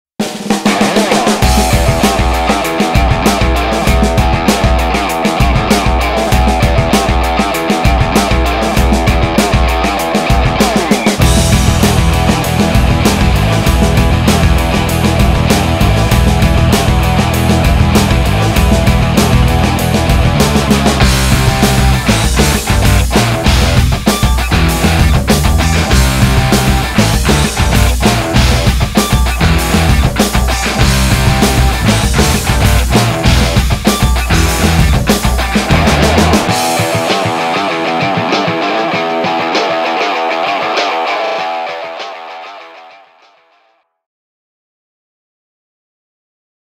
One and half truckers: Shunted by one finger
At the IAA Commercial Vehicles 2014 in Hanover, ZF will present a special innovation: the ZF Innovation Truck.